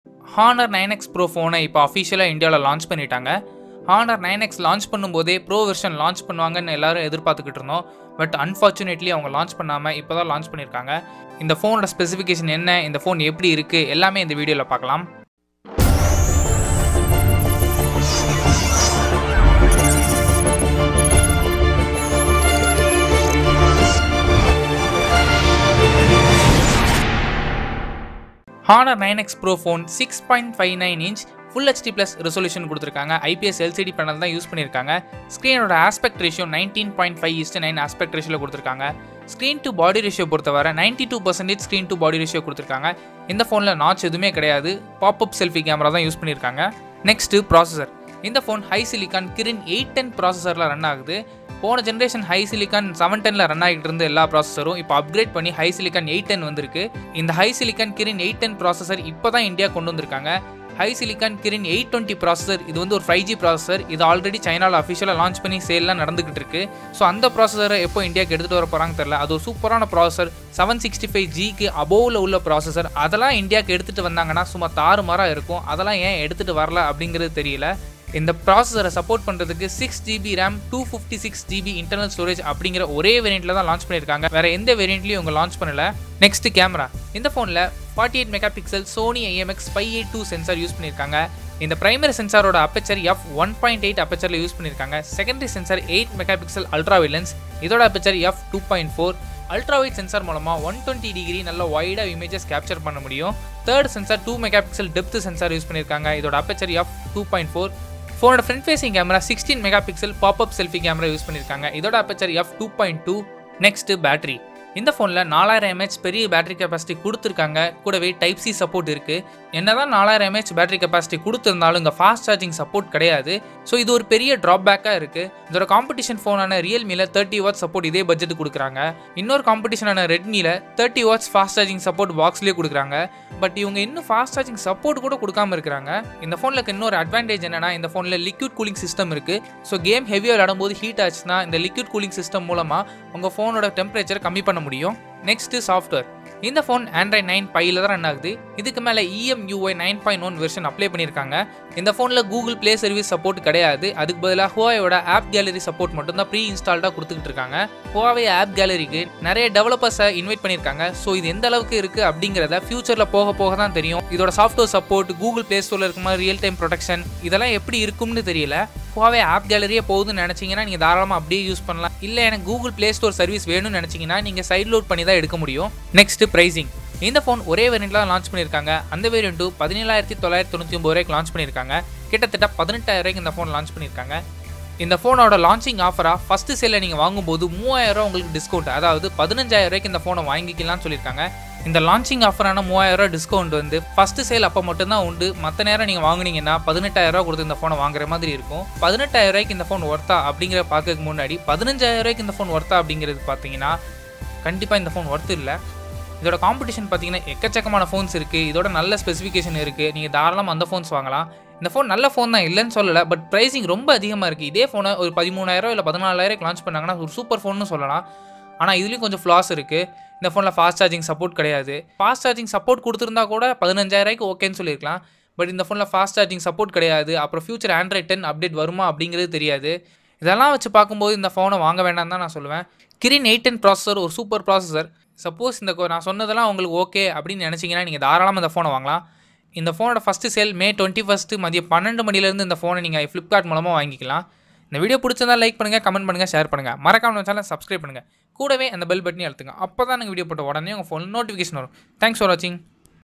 Honor 9X Pro (Kirin 810,6Gb,256Gb,48Mp) For Rs 14,999 Launched In Tamil
UNBOXING,REVIEWS IN TAMIL தமிழ்
SUPPORT US